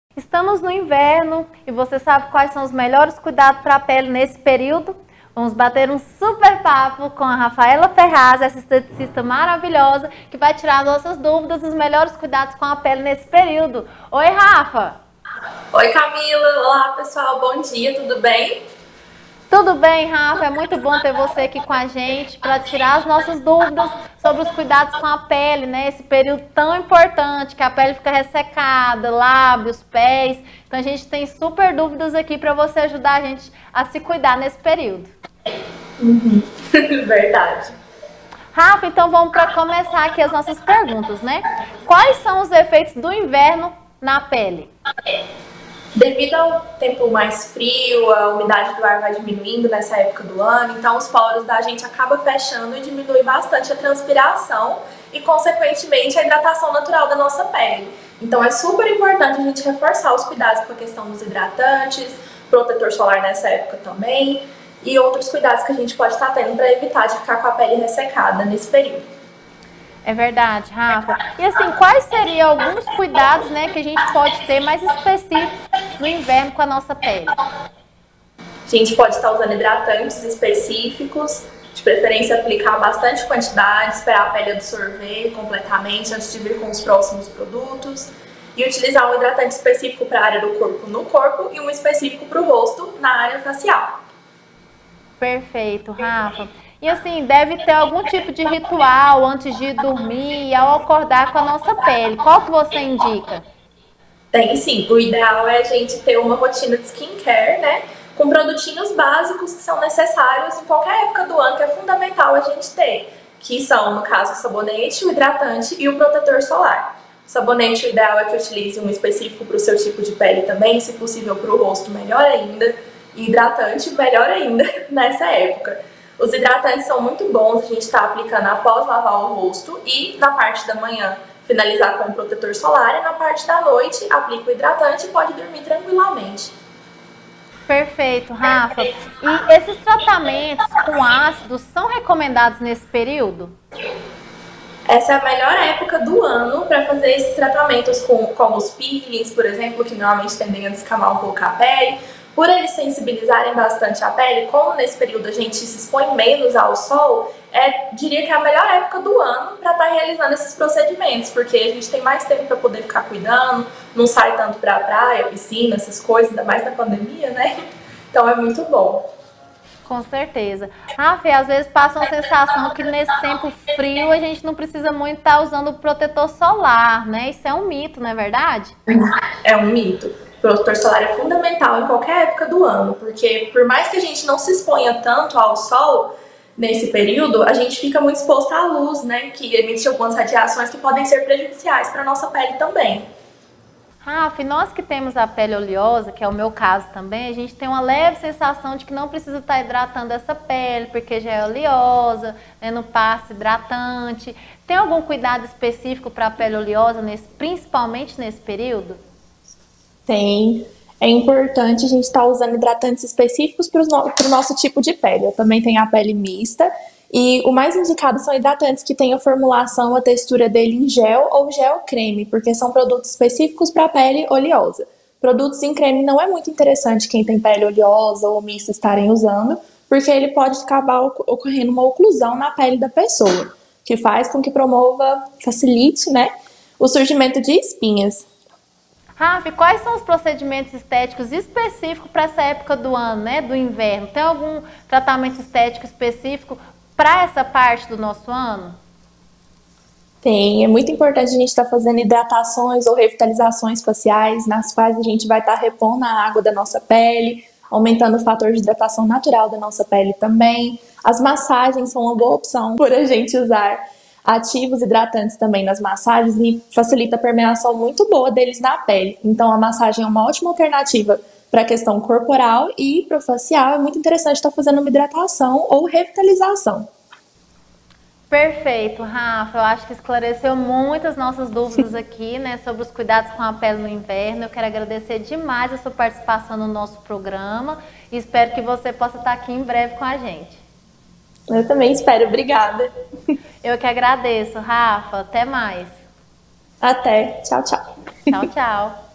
Aprenda a cuidar da pele no inverno com a esteticista Rafaella Ferraz
O inverno está chegando, saiba quais os cuidados que devemos ter com a nossa pele durante esse período.